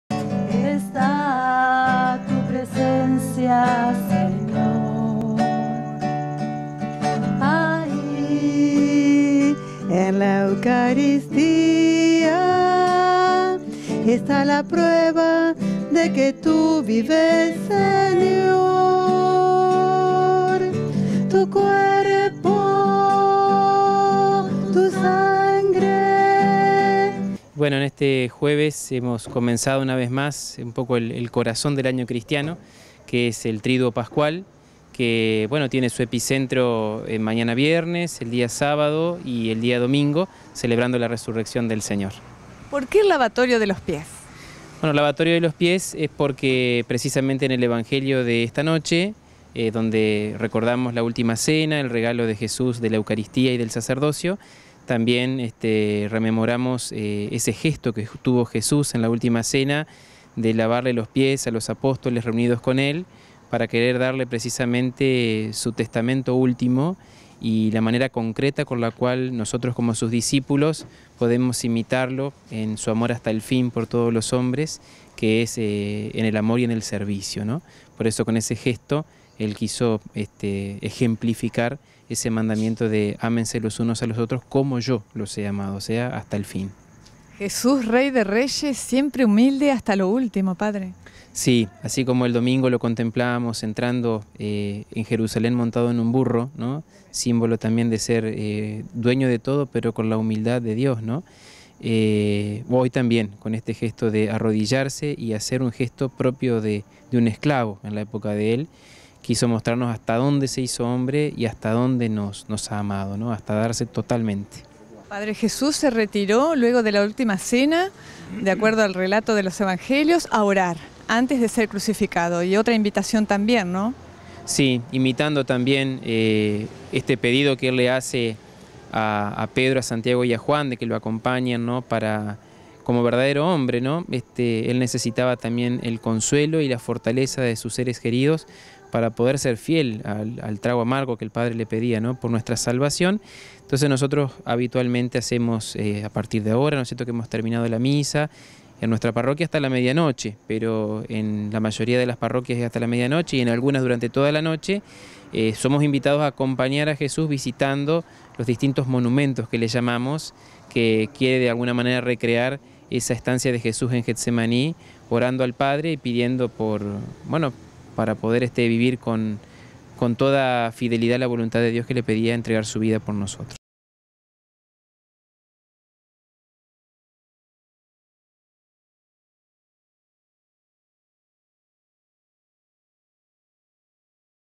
JUEVES SANTO LAVATORIO DE LOS PIES
LA IGLESIA CATÓLICA VIVIÓ EL JUEVES SANTOS
CON DIFERENTES MISAS SE LLEVÓ ADELANTE ESTA TRADICION DONDE SE CELEBRA  LA INSTITUCIÓN DE LA EUCARISTÍA EN LA ÚLTIMA CENA Y EL LAVATORIO DE LOS PIES REALIZADO POR JESÚS.